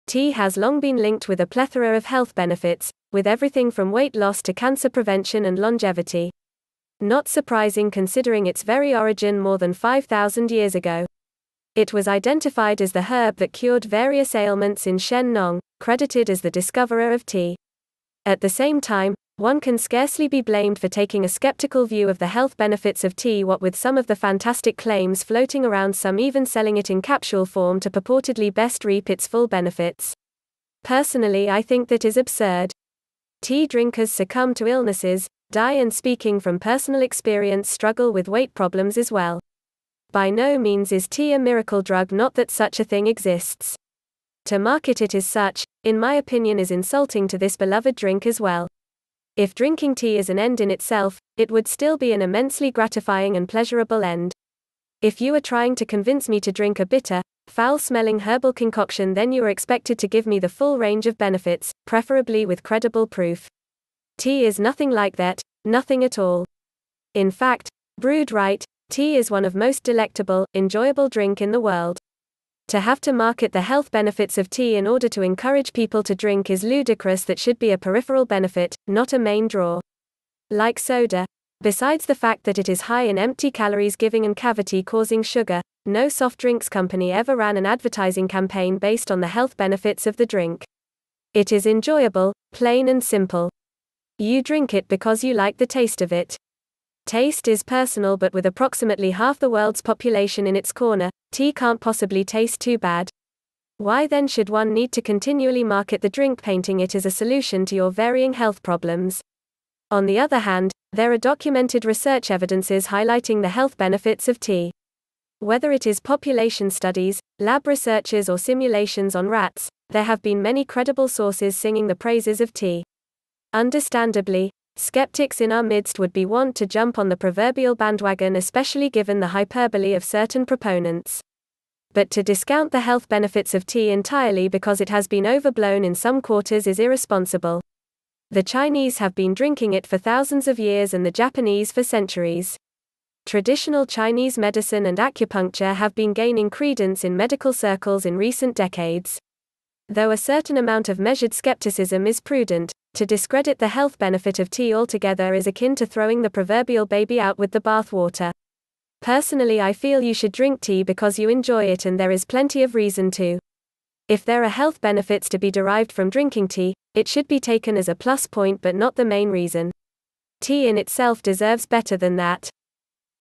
The Health Benefits of Tea - Does It Really Matter?
Tea has long been linked with a plethora of health benefits - with everything from weight loss to cancer prevention and longevity. Not surprising considering its very origin more than 5,000 years ago. It was identified as the herb that cured various ailments in Shen Nong, credited as the discoverer of tea.

At the same time, one can scarcely be blamed for taking a skeptical view of the health benefits of tea- what with some of the fantastic claims floating around- some even selling it in capsule form to purportedly best reap its full benefits.

Personally I think that is absurd. Tea drinkers succumb to illnesses, die and-speaking from personal experience- struggle with weight problems as well. By no means is tea a miracle drug- not that such a thing exists.

To market it as such, in my opinion is insulting to this beloved drink as well. If drinking tea is an end in itself, it would still be an immensely gratifying and pleasurable end. If you are trying to convince me to drink a bitter, foul-smelling herbal concoction then you are expected to give me the full range of benefits, preferably with credible proof.

Tea is nothing like that, nothing at all. In fact, brewed right, tea is one of most delectable, enjoyable drink in the world. To have to market the health benefits of tea in order to encourage people to drink is ludicrous- that should be a peripheral benefit, not a main draw.

Like soda - besides the fact that it is high in empty calories giving and cavity causing sugar - no soft drinks company ever ran an advertising campaign based on the health benefits of the drink. It is enjoyable, plain and simple. You drink it because you like the taste of it.

Taste is personal but with approximately half the world's population in its corner, tea can't possibly taste too bad. Why then should one need to continually market the drink painting it as a solution to your varying health problems?

On the other hand, there are documented research evidences highlighting the health benefits of tea. Whether it is population studies, lab researches or simulations on rats, there have been many credible sources singing the praises of tea.

Understandably, skeptics in our midst would be wont to jump on the proverbial bandwagon- especially given the hyperbole of certain proponents. But to discount the health benefits of tea entirely because it has been overblown in some quarters is irresponsible.

The Chinese have been drinking it for thousands of years and the Japanese for centuries. Traditional Chinese Medicine and acupuncture have been gaining credence in medical circles in recent decades. Though a certain amount of measured skepticism is prudent, to discredit the health benefit of tea altogether is akin to throwing the proverbial baby out with the bath water.

Personally I feel you should drink tea because you enjoy it- and there is plenty of reason to. If there are health benefits to be derived from drinking tea, it should be taken as a plus point but not the main reason. Tea in itself deserves better than that.